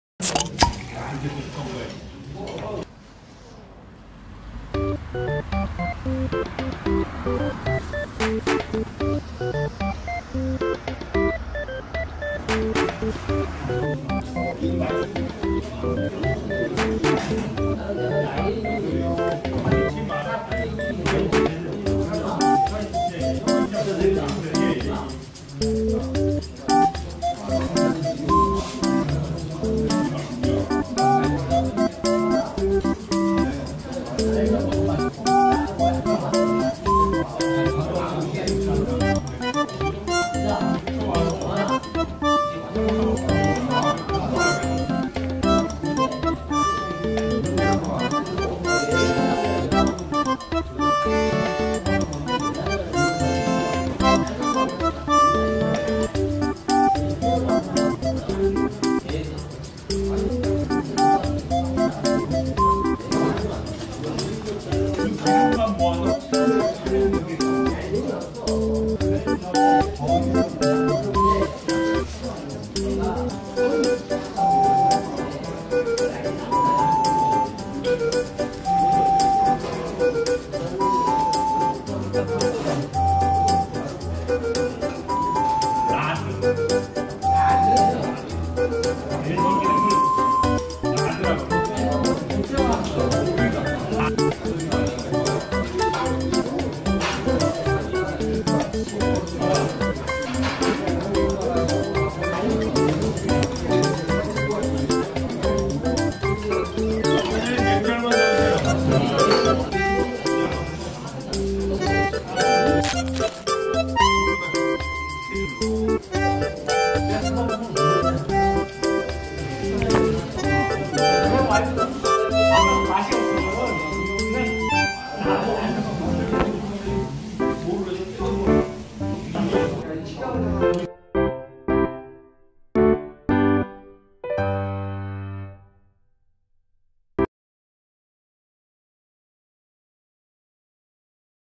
오삼불고기 평창 # 맛의 기억Korean food
Osam Bulgogi I ate while traveling
It was really delicious.
If you go to Pyeongchang, please try it.